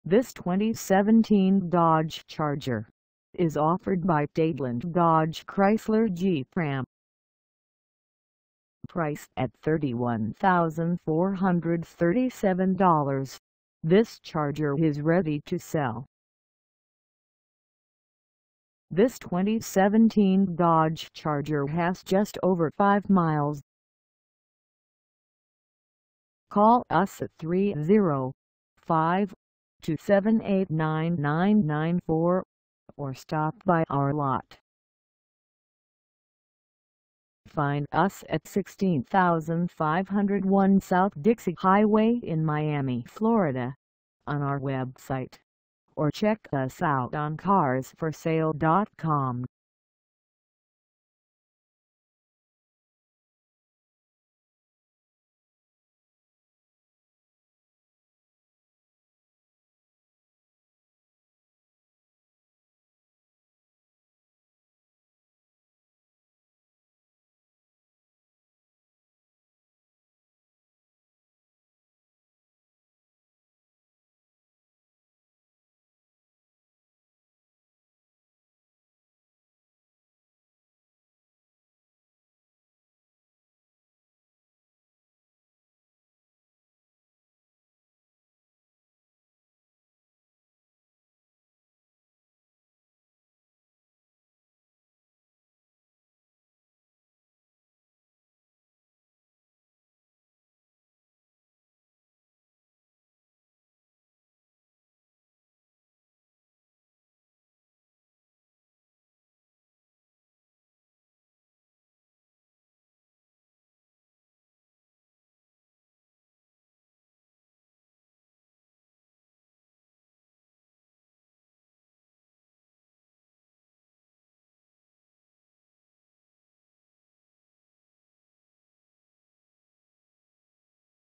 2017 Dodge Charger SXT 4dr Sedan for sale in Miami, FL 33157
This 2017 Dodge Charger SXT 4dr Sedan is for sale in Miami, FL 33157 at DADELAND DODGE CHRYSLER JEEP RAM.

Engine: 3.6L V6
Transmission: Automatic 8-Speed
Color: MAXIMUN_STEEL
Mileage: 5
Contact DADELAND DODGE CHRYSLER JEEP RAM 16501 South Dixie Highway Miami, FL 33157 to test drive this 2017 Dodge Charger SXT 4dr Sedan today.